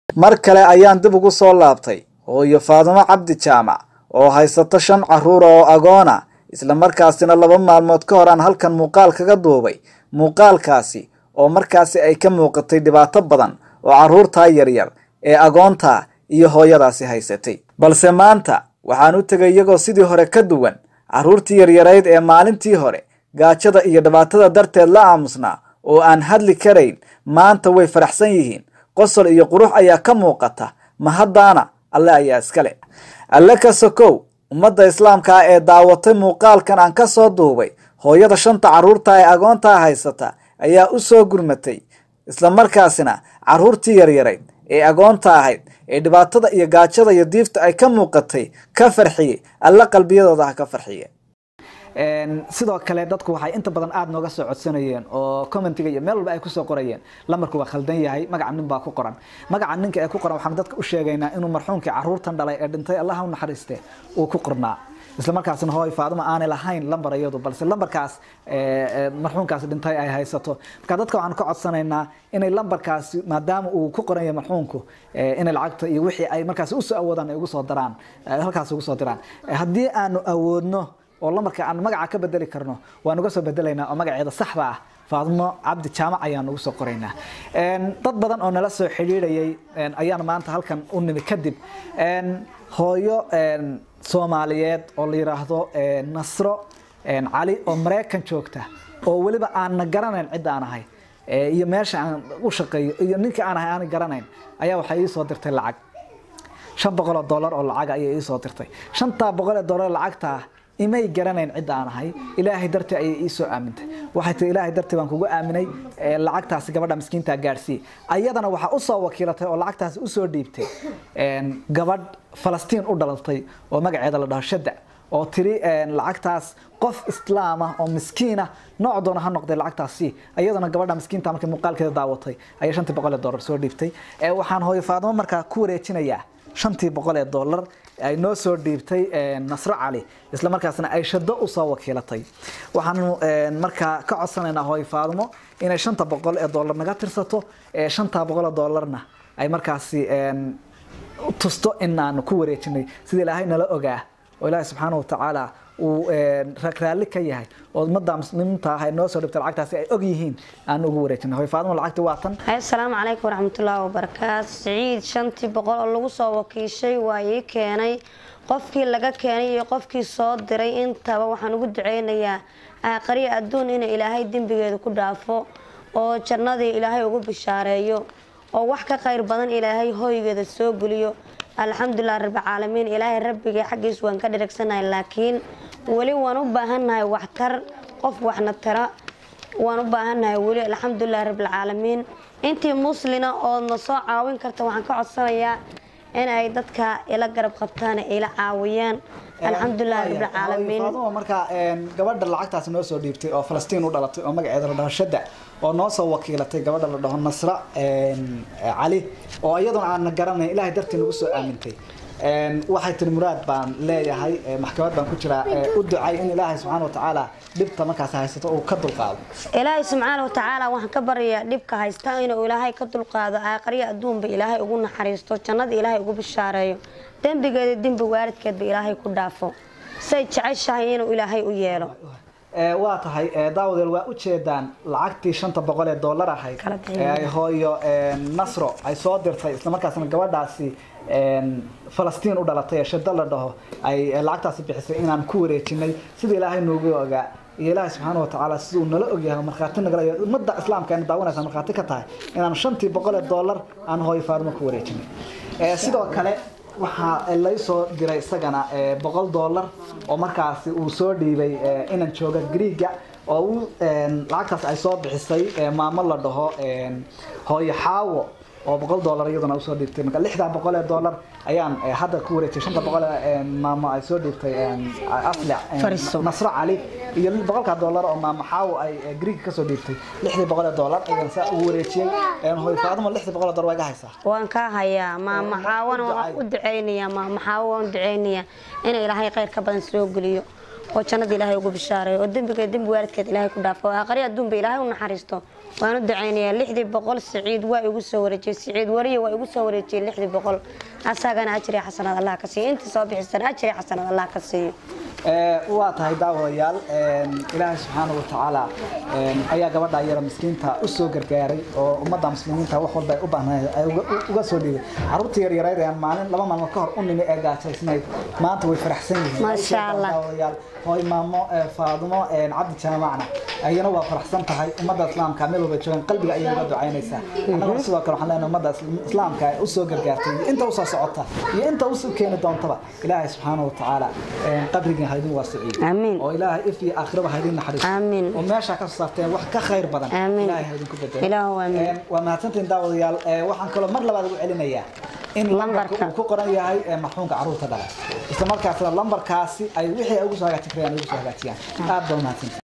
DAAWO"Gabar u dhalatay Palestine ayaa 500 $ uu soo dirtay Hooyo Faadumo Cabdi Jaamac
Hooyo Faadumo Cabdi Jaamac, oo ahayd hooyo shan caruur ah oo Agoon haysatay oo aan maalmo ka hor muqaal ka soo dubay ayaan maanta ugu laabtay,

fiiro gar ah magaca ku qoran telka waa magaca marxuunka caruurta dhalay,